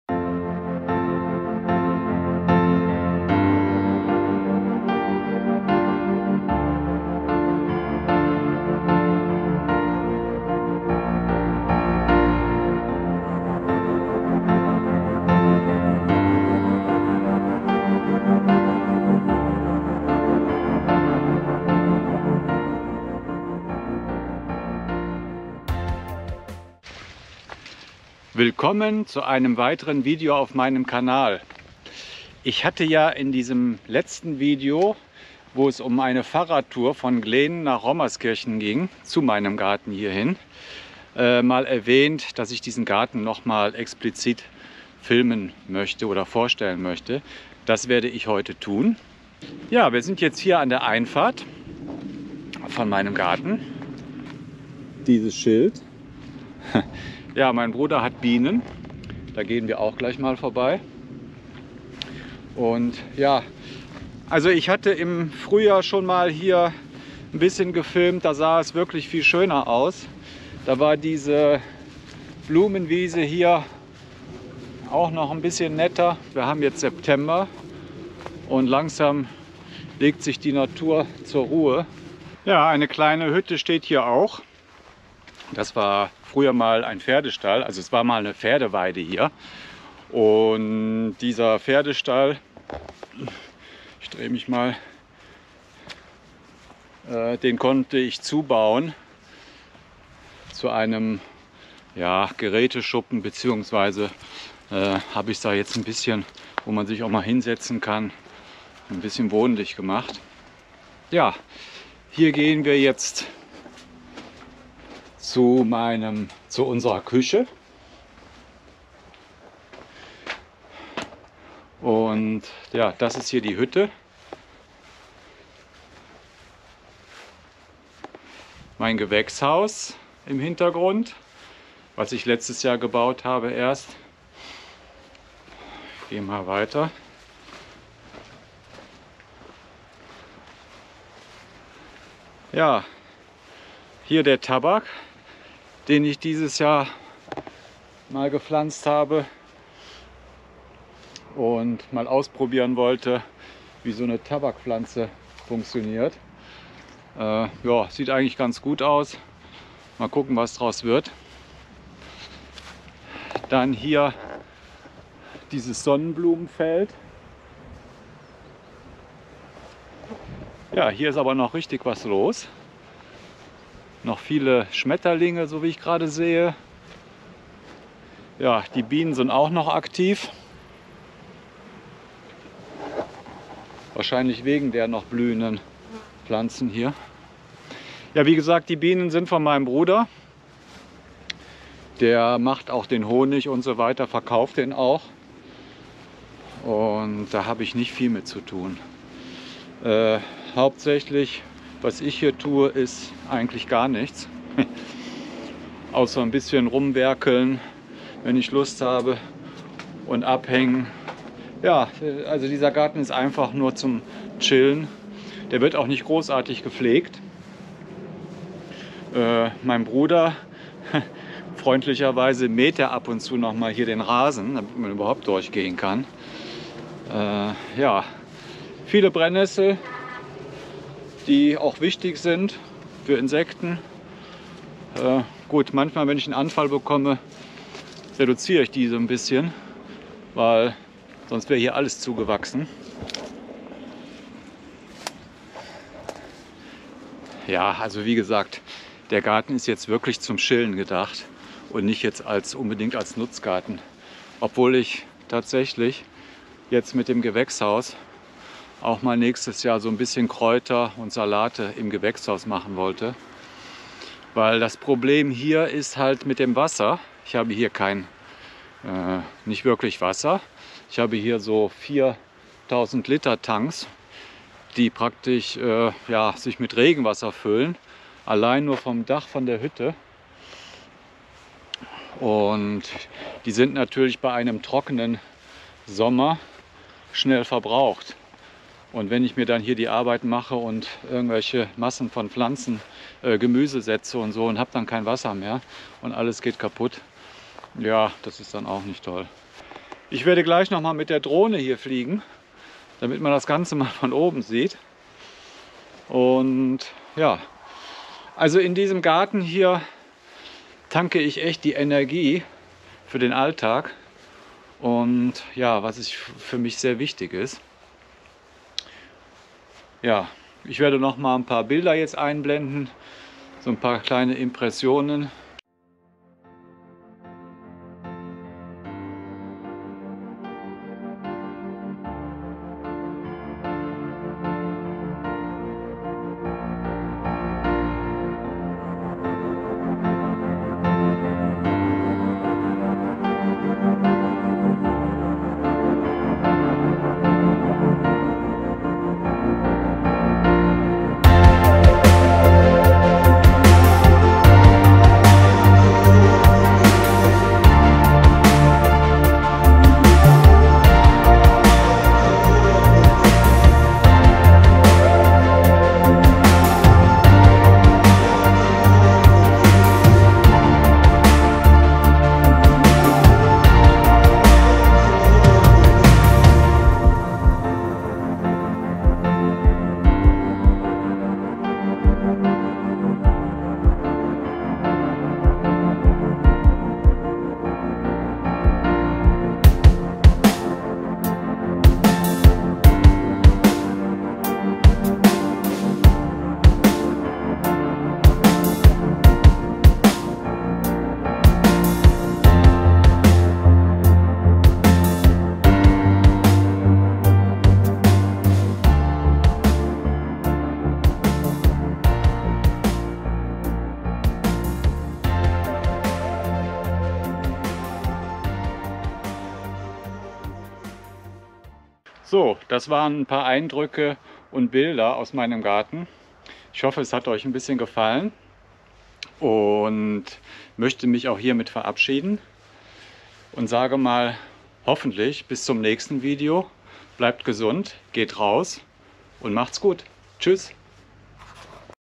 Mein Garten kurz vorgestellt
In dieser kleinen Oase tanke ich viel Kraft für den Alltag.